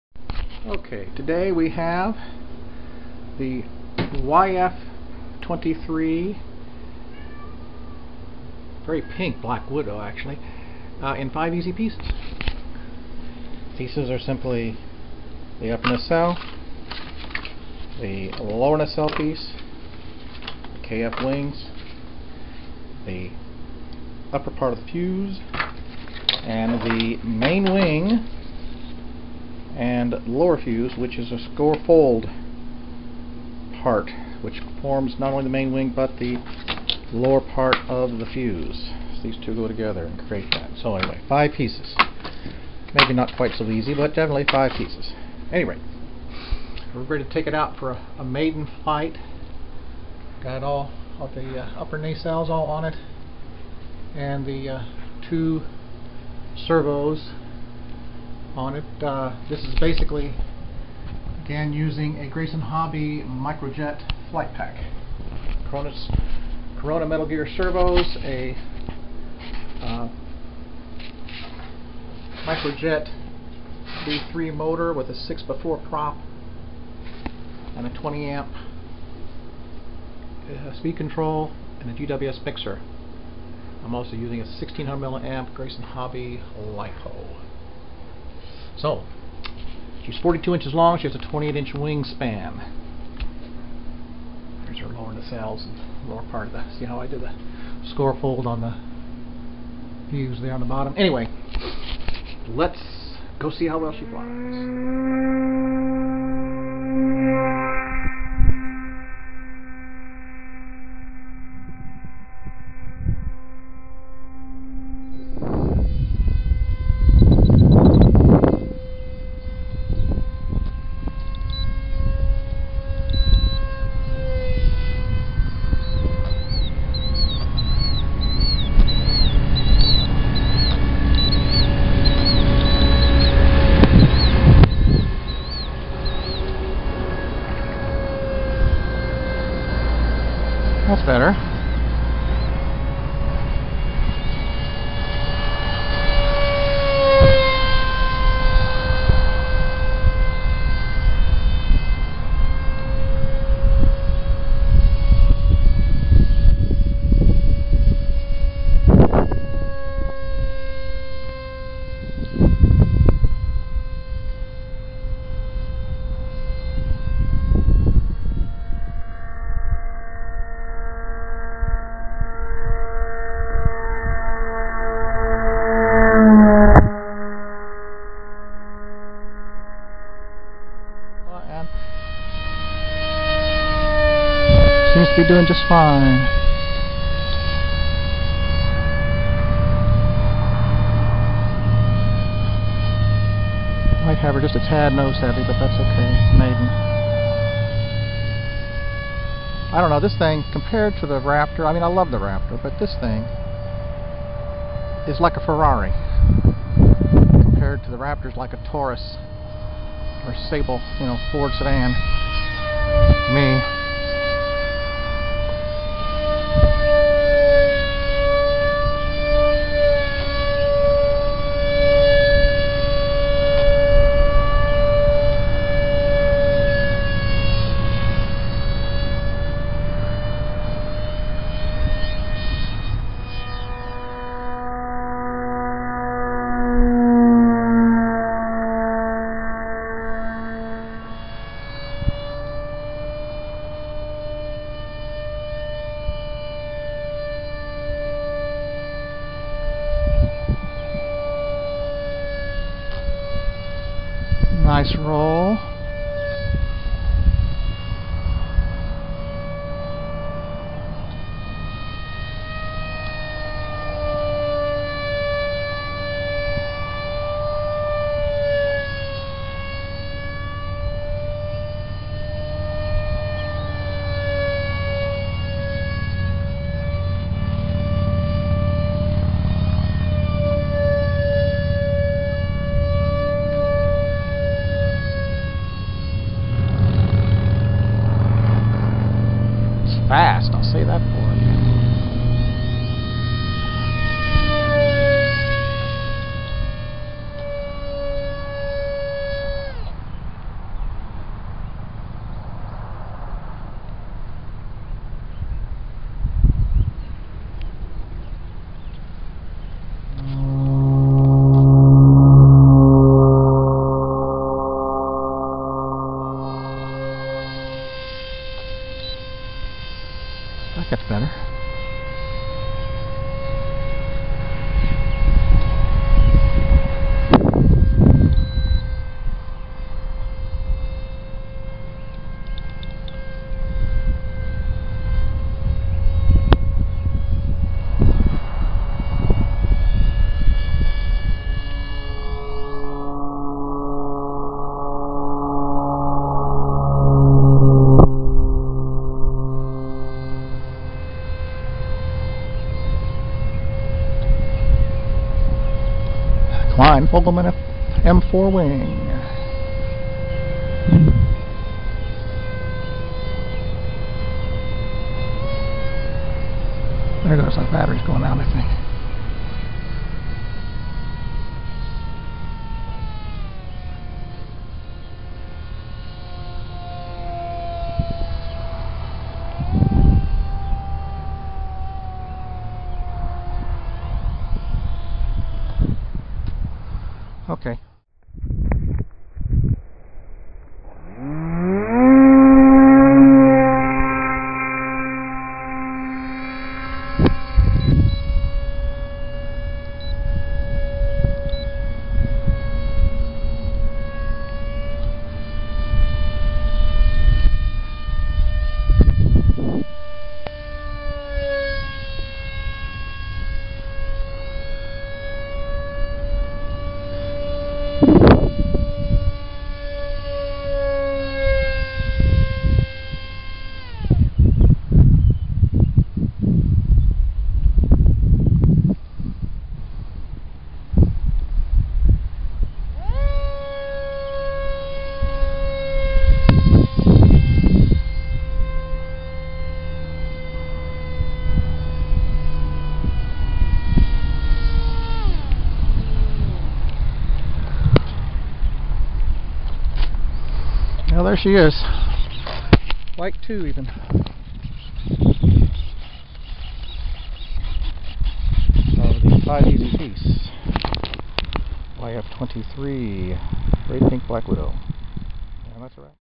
Five Easy Piece YF-23 Maiden Flight .mov
Completed the build and installed the gizmos this morning then took her out at noon for her maiden flight.
She is a three dimensional scratch-built foam park-jet 42" long with a 28" Kline-Fogleman wingspan and is equipped with a Grayson Hobby Micro-jet Flight-Pack.